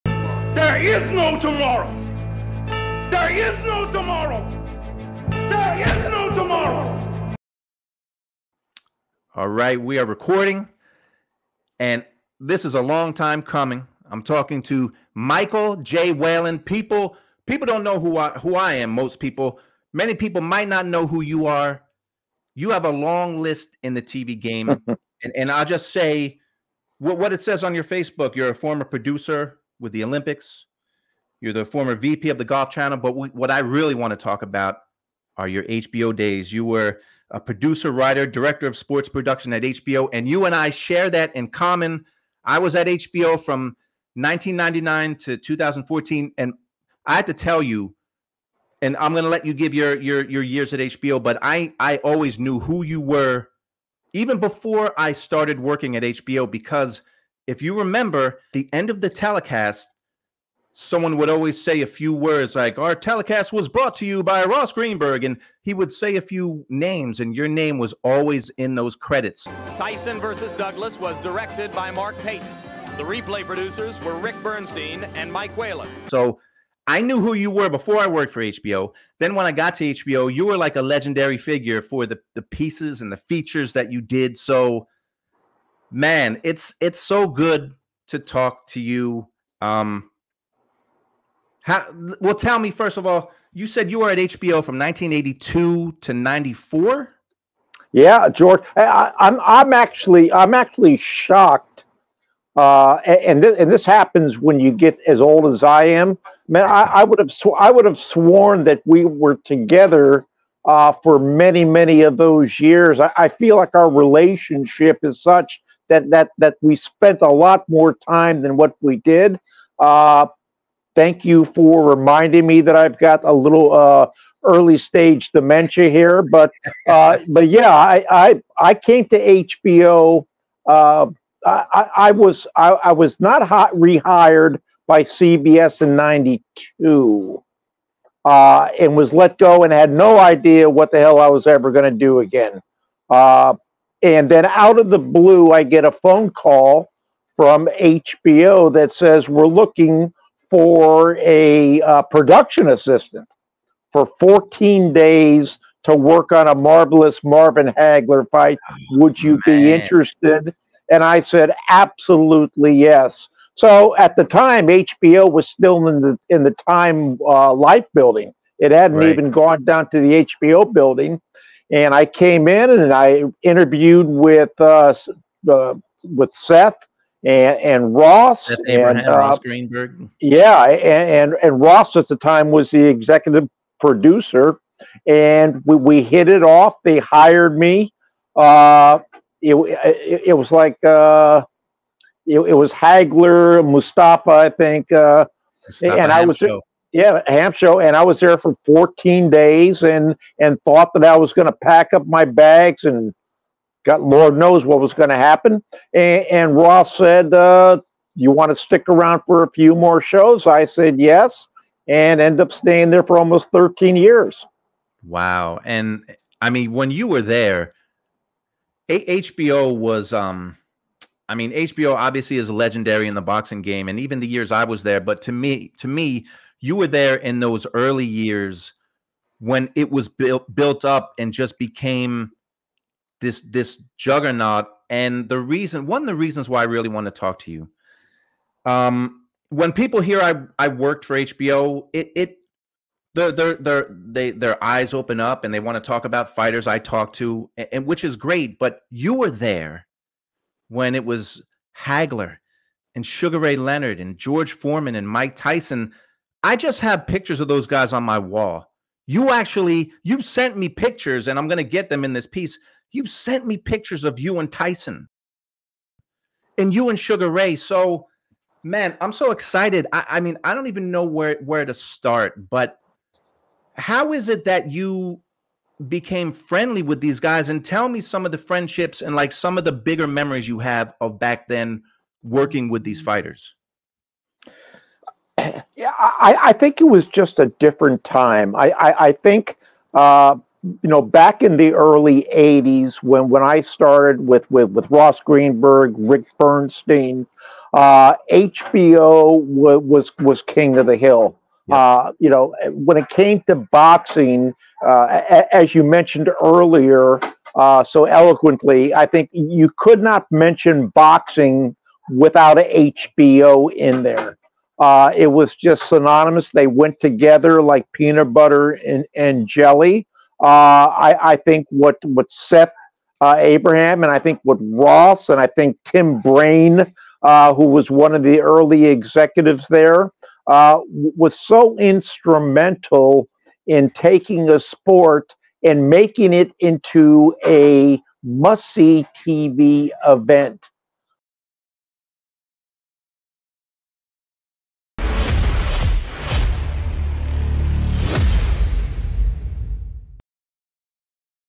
WHEN HBO WAS KING...The Producer Chronicles Preview - 7:15 PM TONIGHT
-Been waiting 3 years for this.   This is my conversation with former HBO Producer Michael J. Whelan.
-Mike was at HBO from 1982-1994, when HBO was the King Of Boxing.  He forged frienships with Mike Tyson, Sugar Ray Leonard, Hector Camacho and more at a time when no network was bigger than HBO Sports.
-Mike is also a cancer survivor, still battling the disease.  This is a 3 part story about boxing and his time at HBO.
-Enjoy this short preview, more to come.